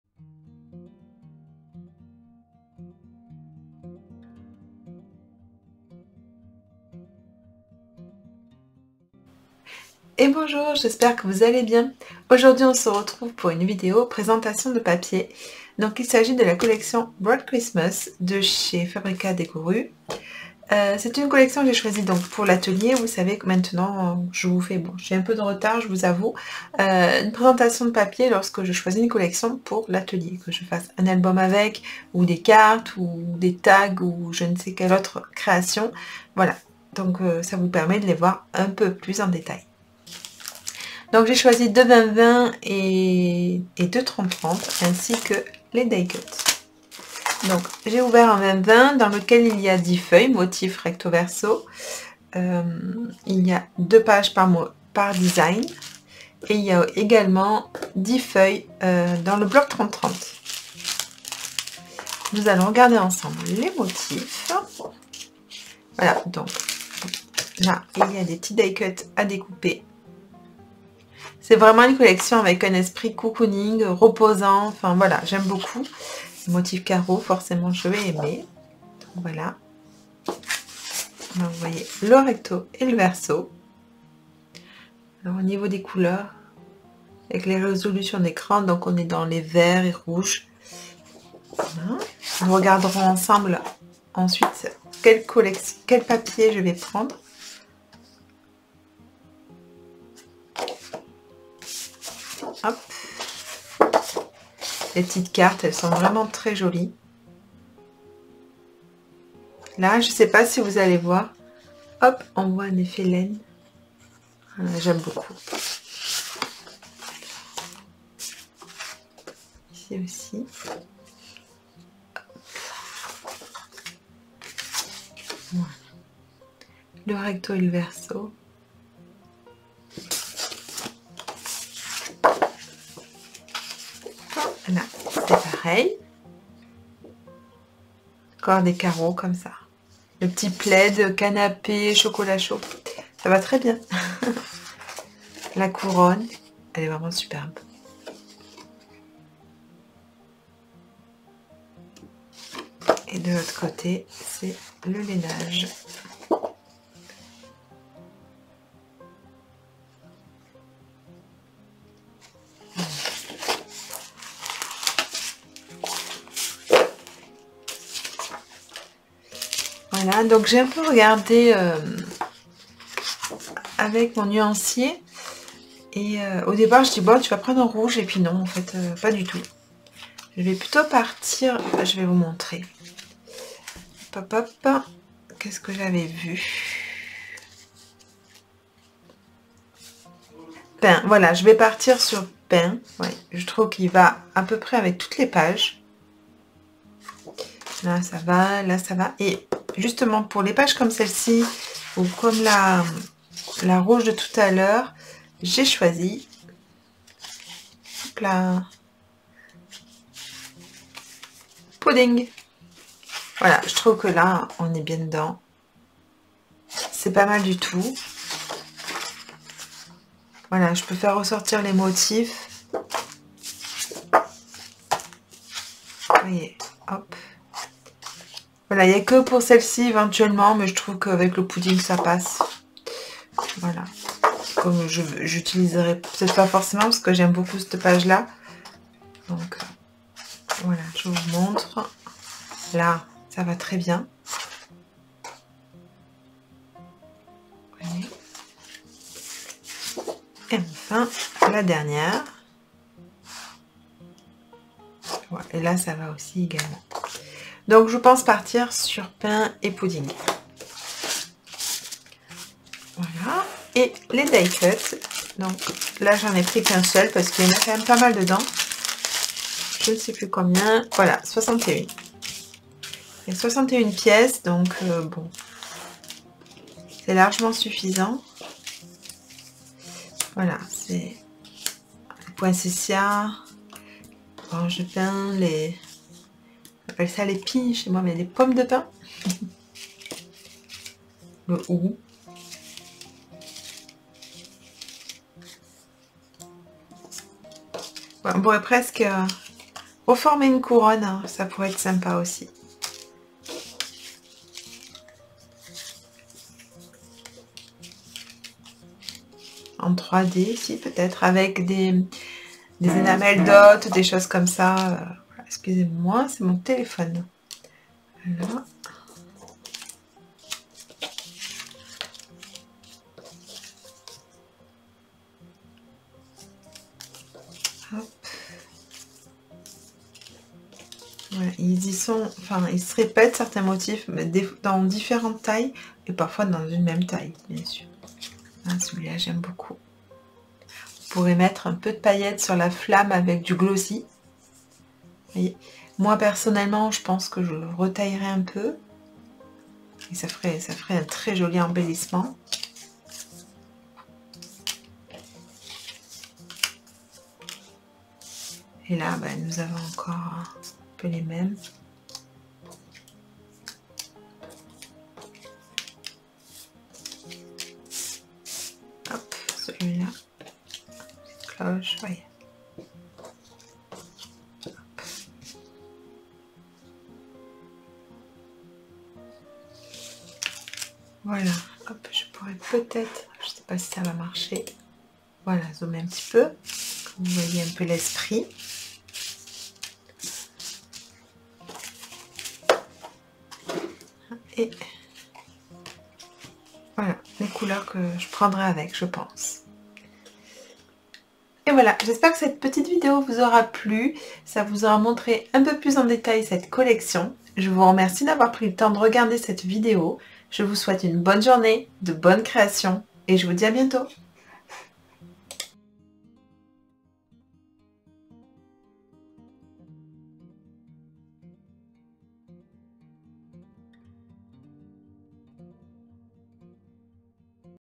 [PRESENTATION COLLECTION PAPIERS] Bright Christmas
!! Pensez à dérouler !!!

Bonjour,
Je vous présente une collection de papier Bright christmas
Ninon

horizon_creatif